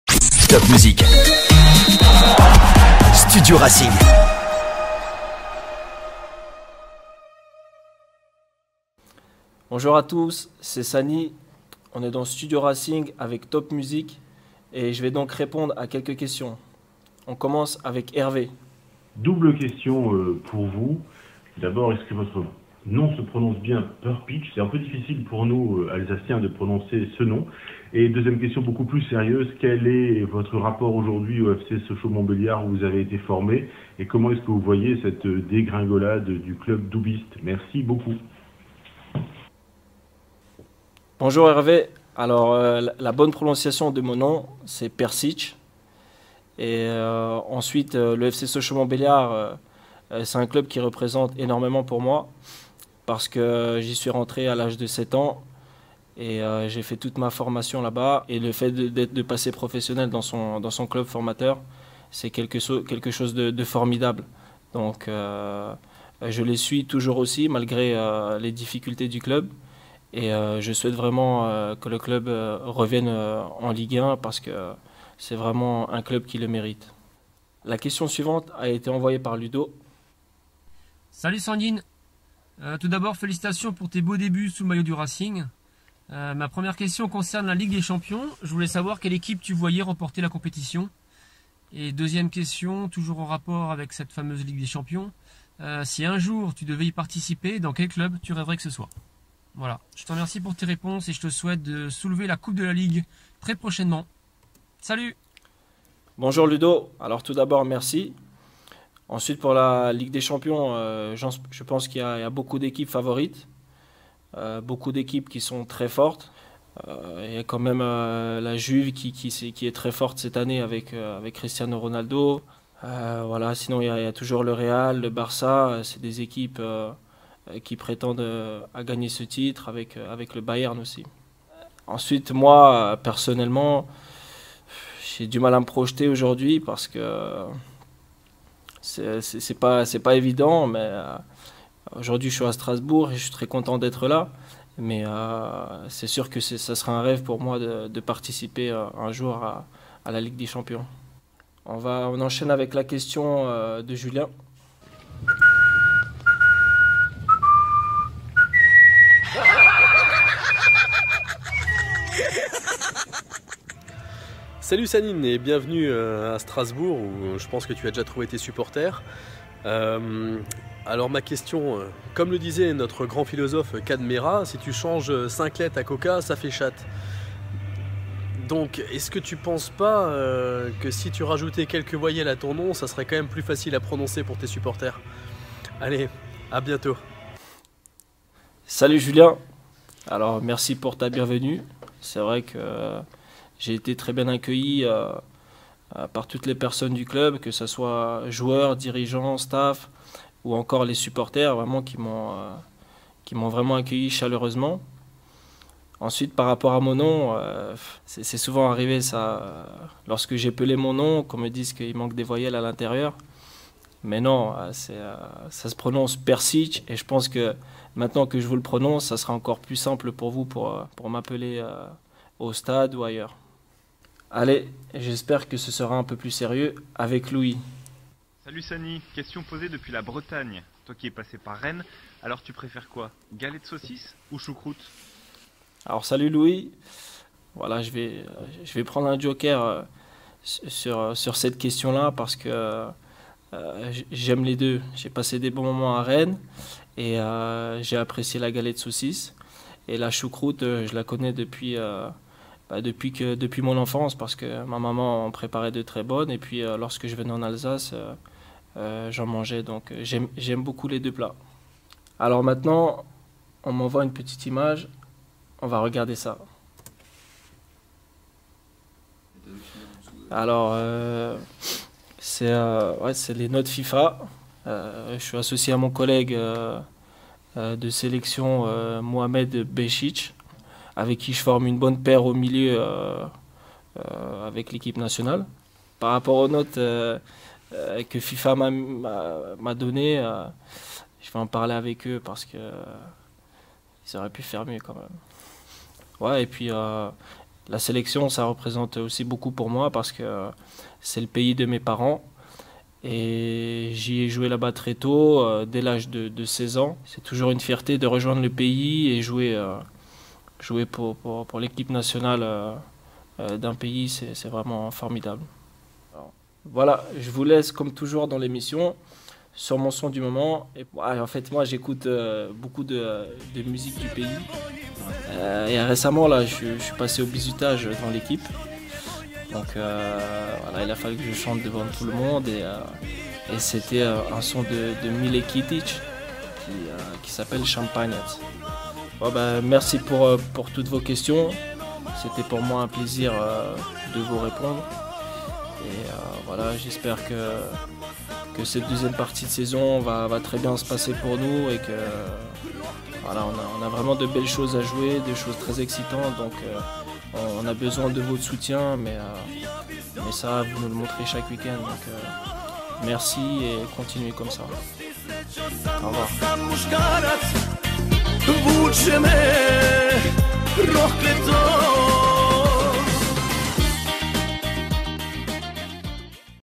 Studio Racing / Sanjin Prcic
Studio Racing, le rendez-vous des fans du Racing.
Une émission en partenariat entre Top Music et le RCSA.
Dans cet épisode, le milieu de terrain Sanjin Prcic.